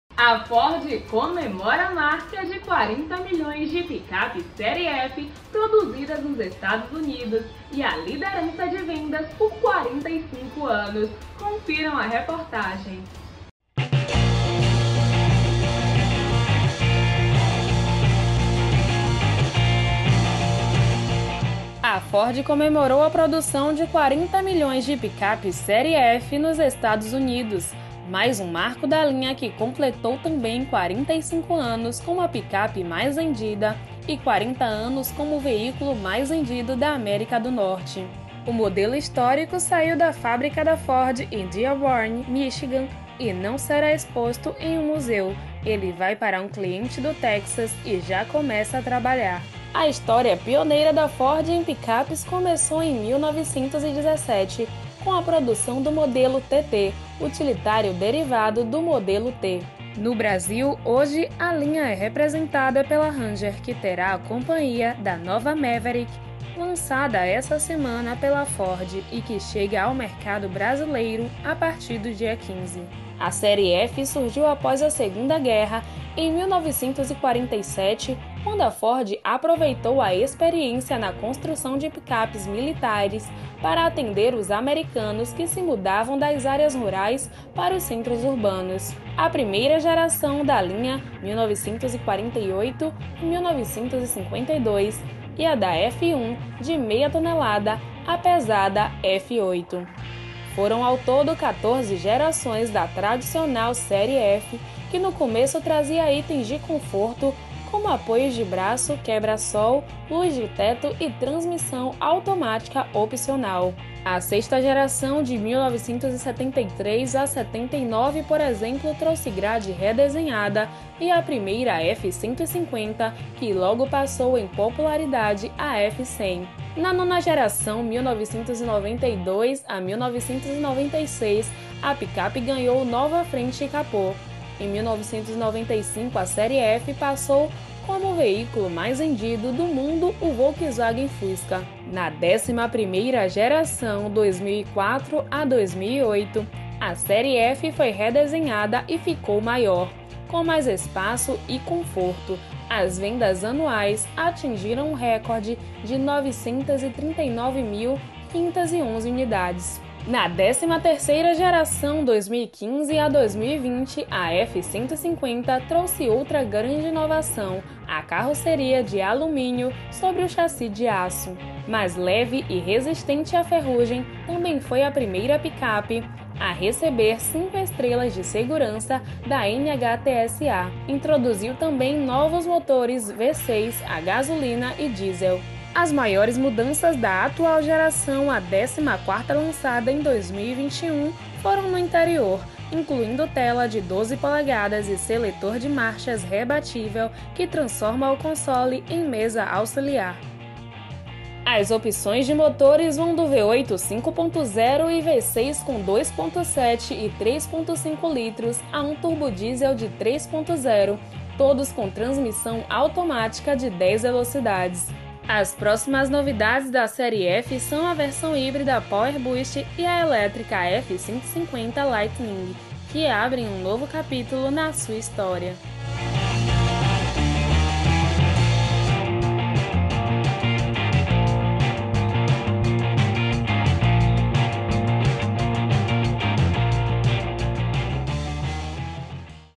Ford comemora a produção de 40 milhões de picapes da Série F
F1502023